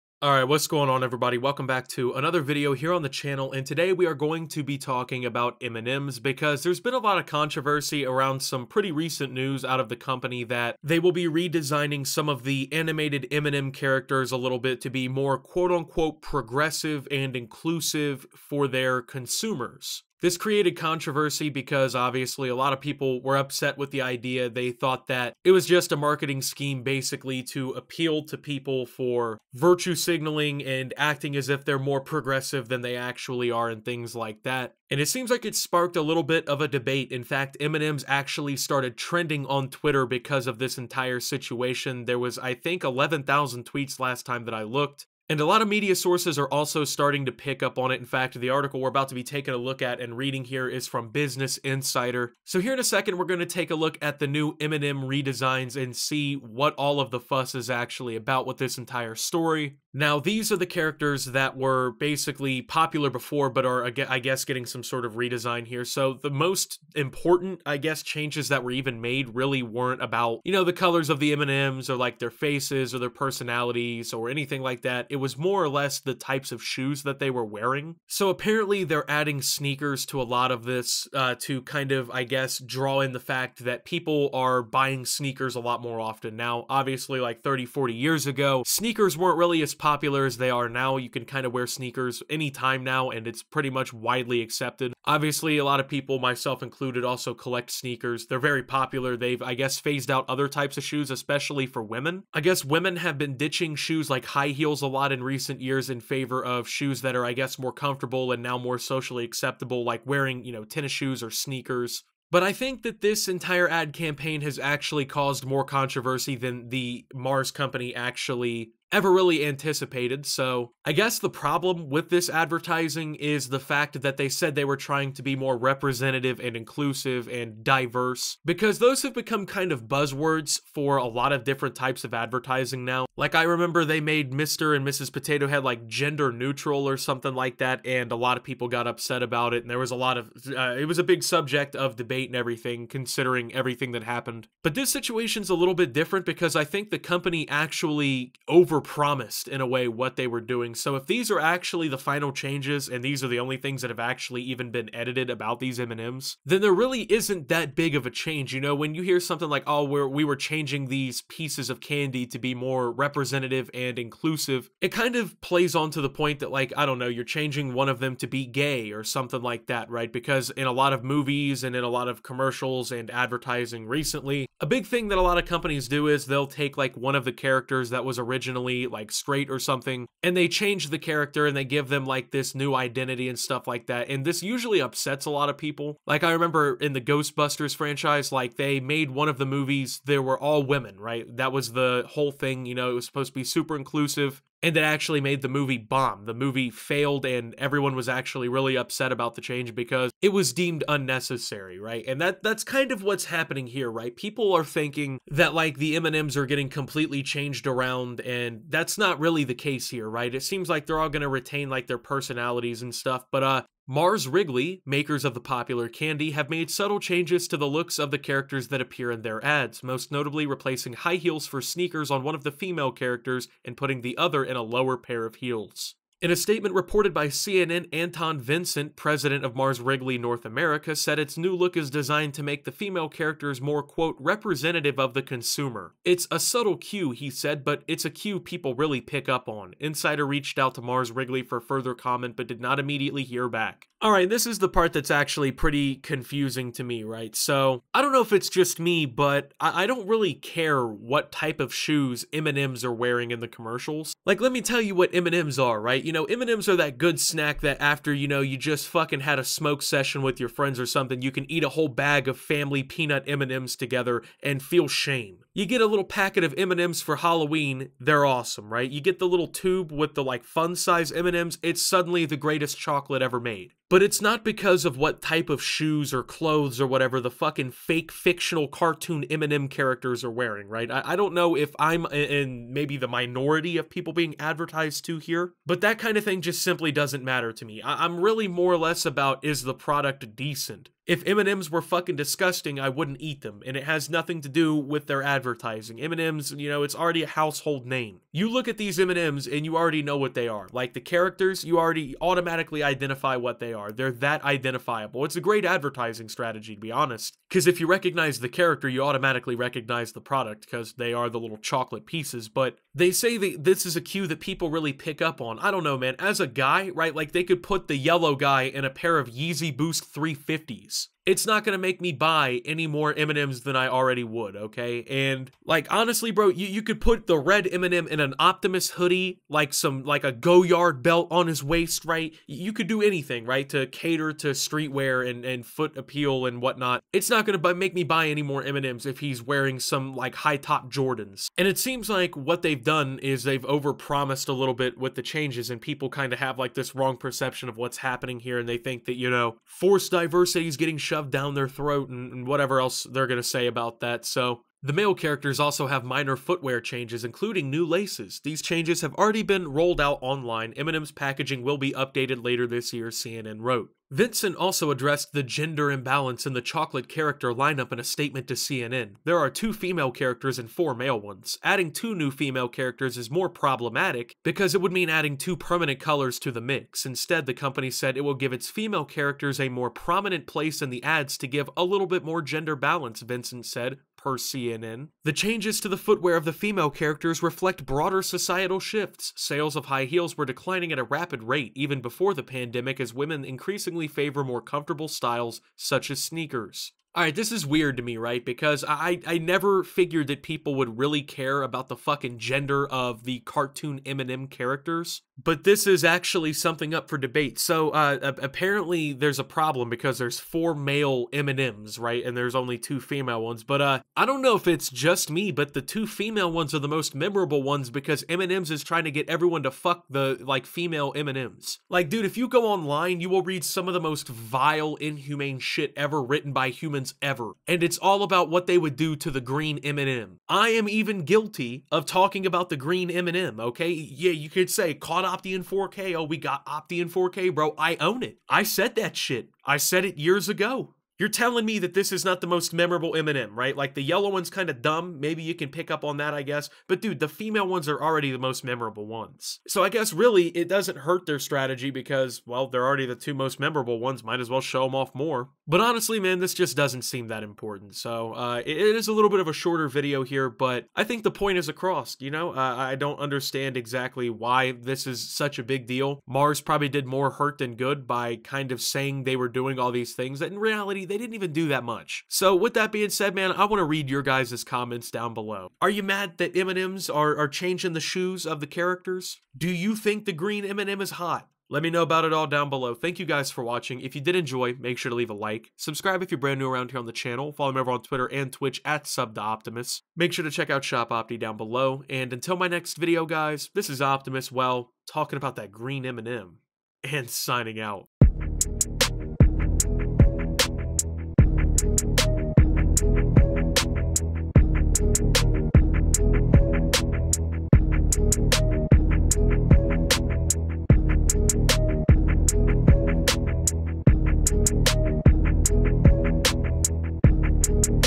M&M's Redesigned To 'Be More Inclusive'
M&M's have announced that they've redesigned their ads to be more 'inclusive.'